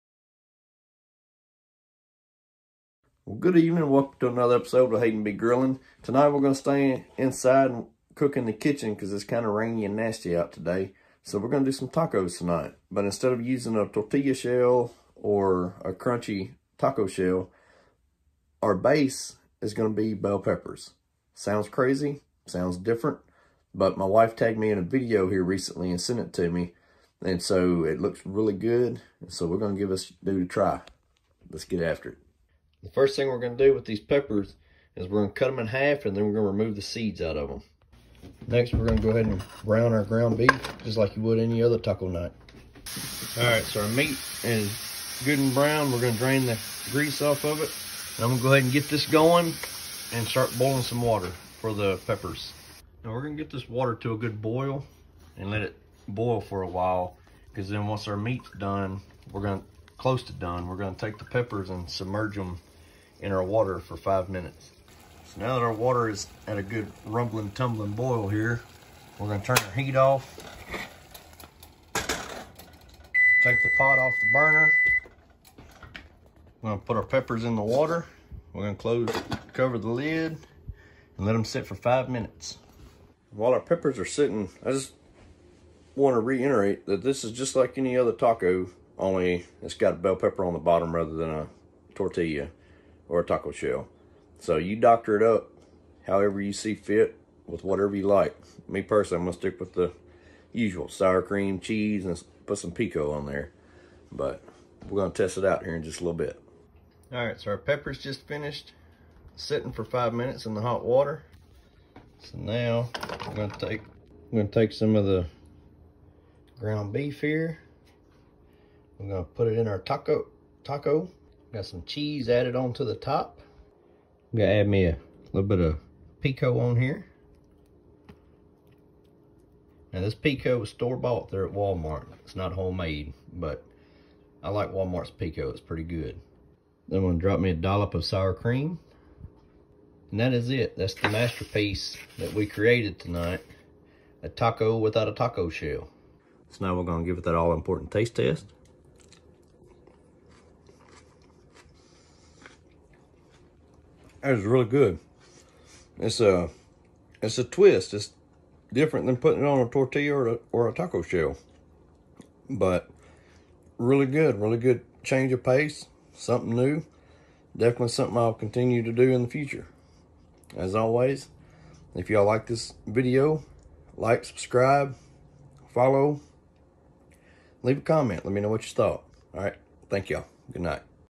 Taco Night With A Twist!!!!
Whole new way of making taco night even better. I never would have dreamt of putting tacos on a bell pepper if it hadn’t been for my wife tagging me in a video. We both really enjoyed tonight’s meal. Please give this a try!